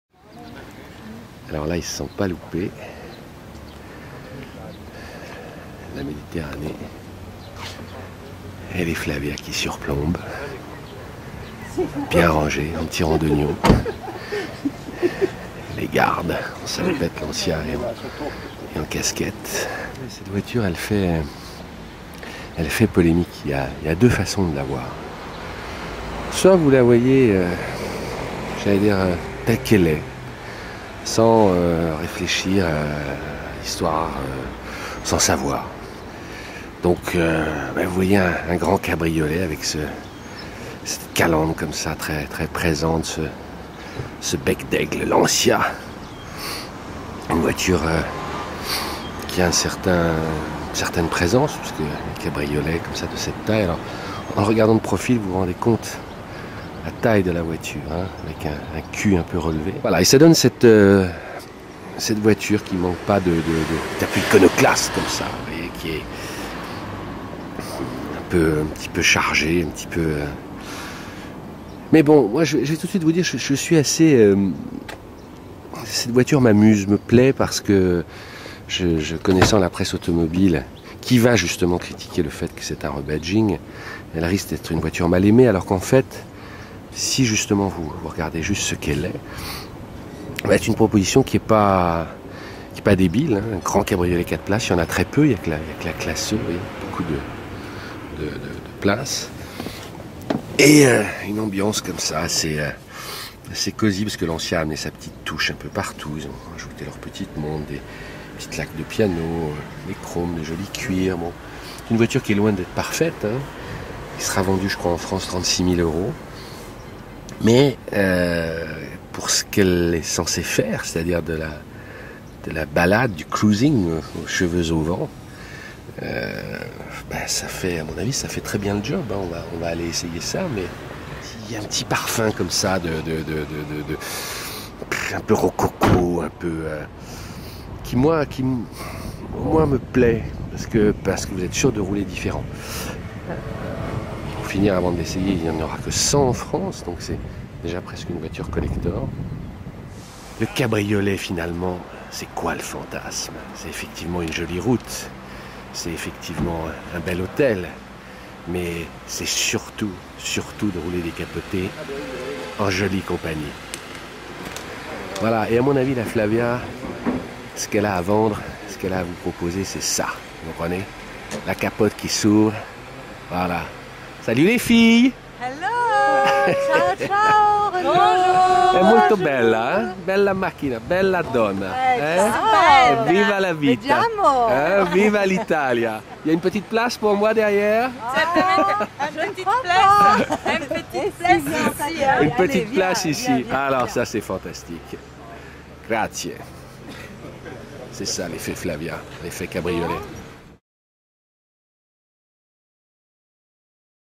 Lancia Flavia Cabriolet à l'essai sur les routes de Sicile
Renaud Roubaudi de Petites Observations Automobile, nous présente la Lancia Flavia 2012 à Taormina en Sicile (part 1).Découvrez Petites Observations Automobile